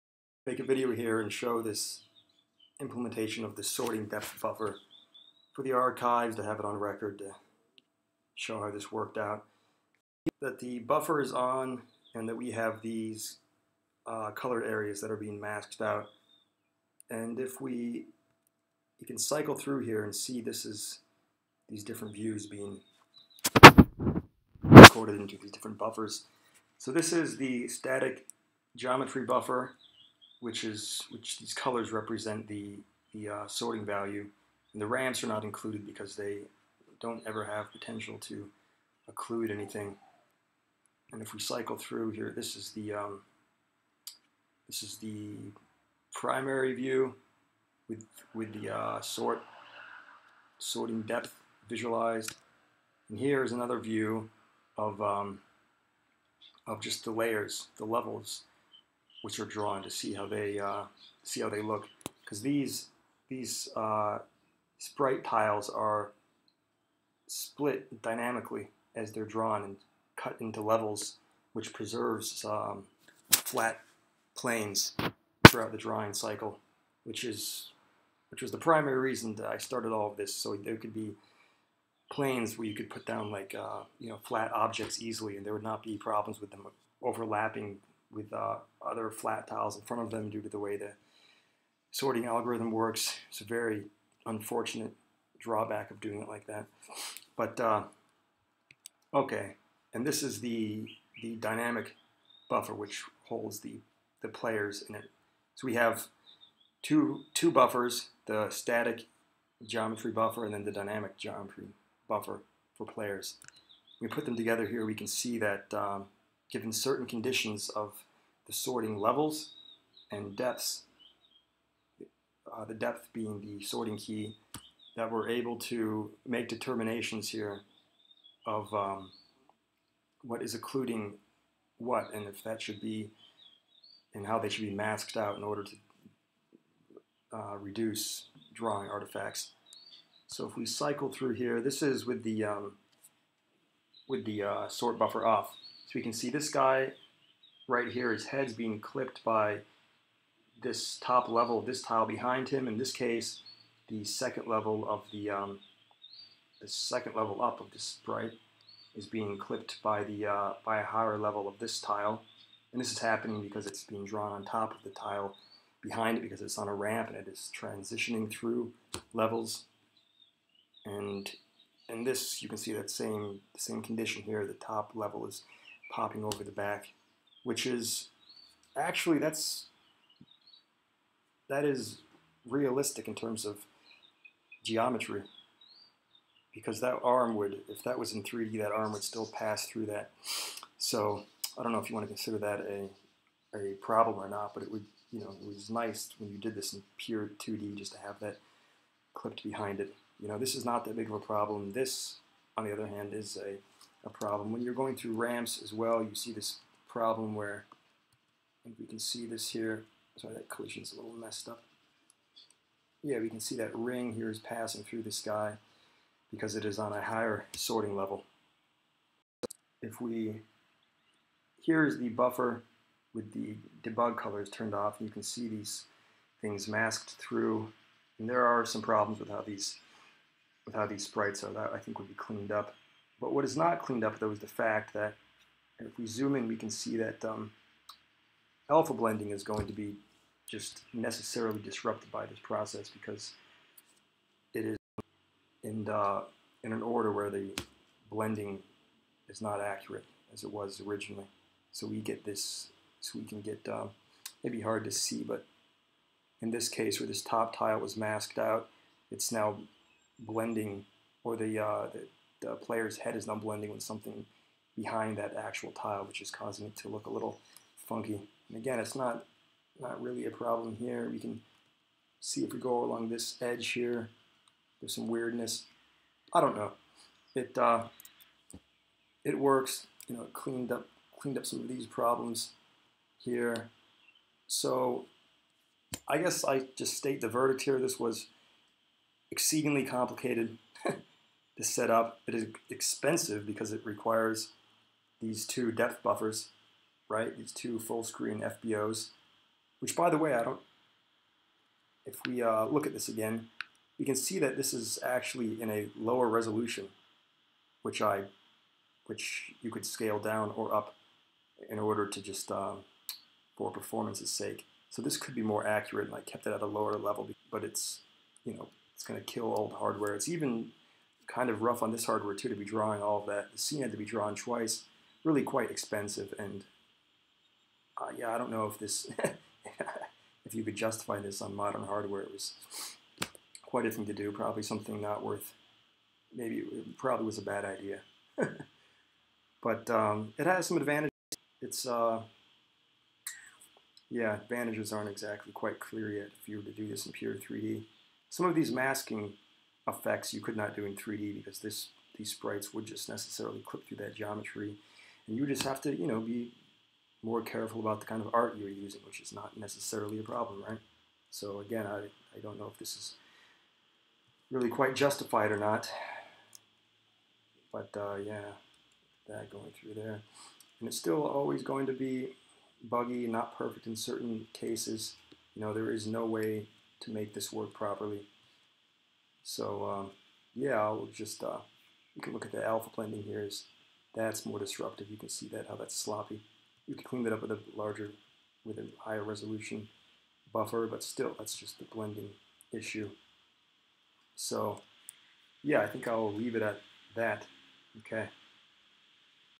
Isometric Sorting Depth Buffer Implementation (OpenGL)
Follow up implementation to the isometric sorting depth buffer proposal I made a few weeks ago.